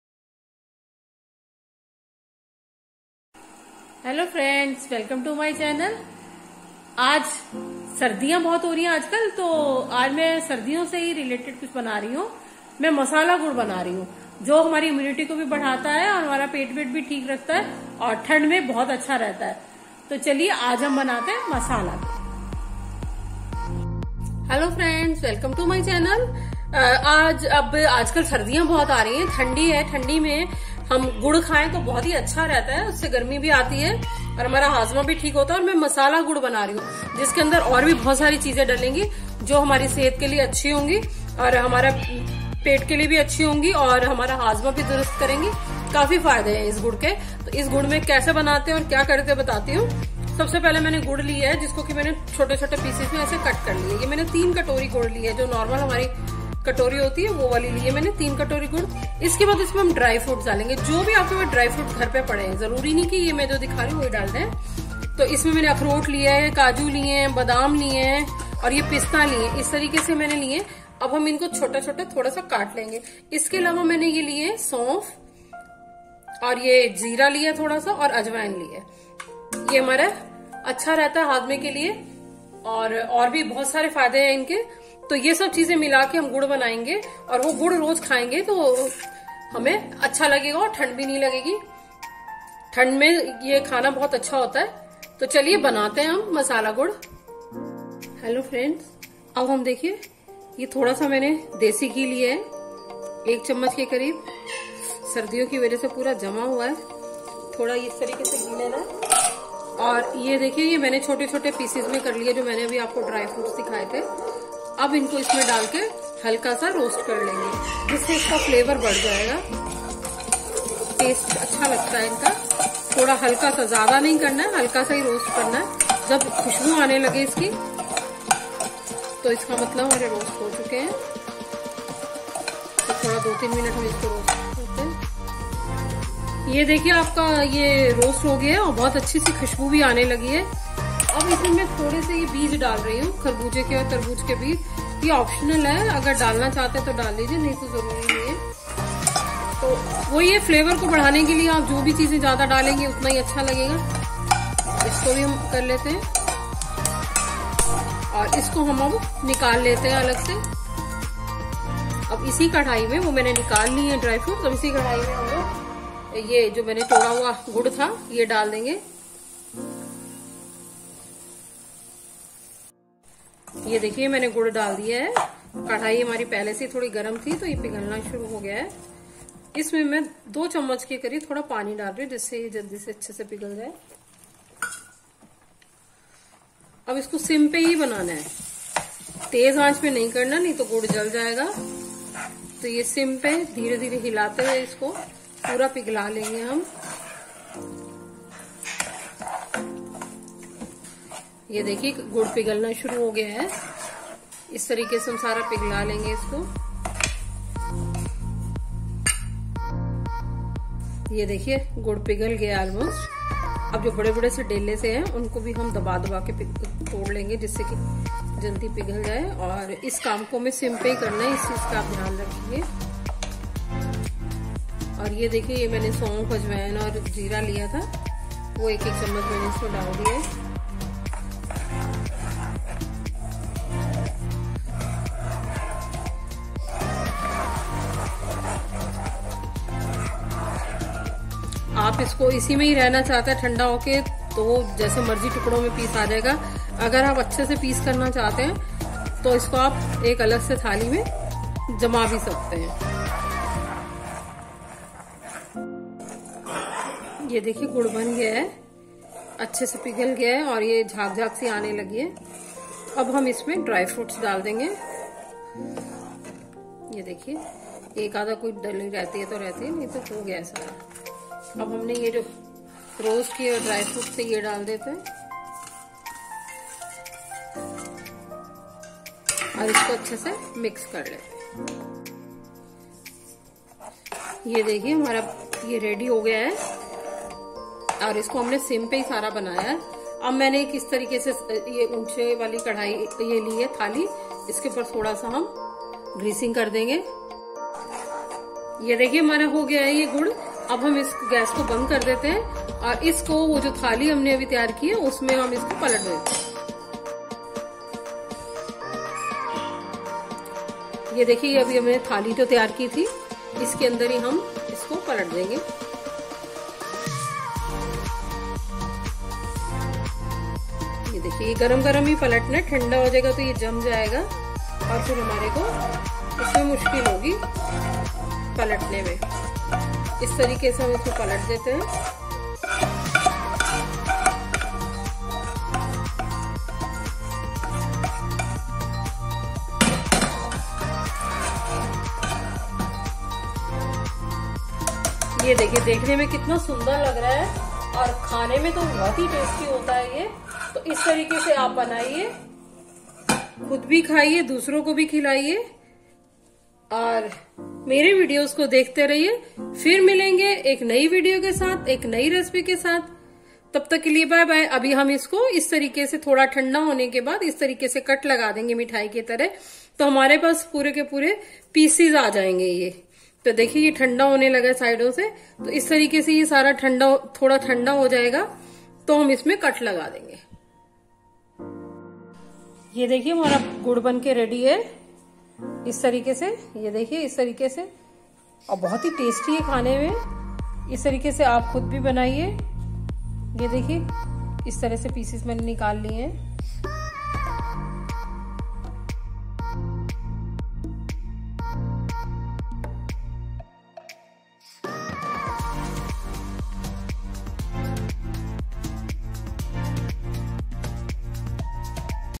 How To Make Masala Gur | Easy Masala Gur Recipe/KRC creation
How To Make Masala Gur | Easy Masala Gur Recipe
Masala Gur (Masala Jaggery) Healthy & Immunity Booster I मसाला गुड़
JAGGERY/GUR/ MASALA -GUR/SPICED-JAGGERY/ANTI - POLLUTION /HEALTHY /EASY /HOMEMADE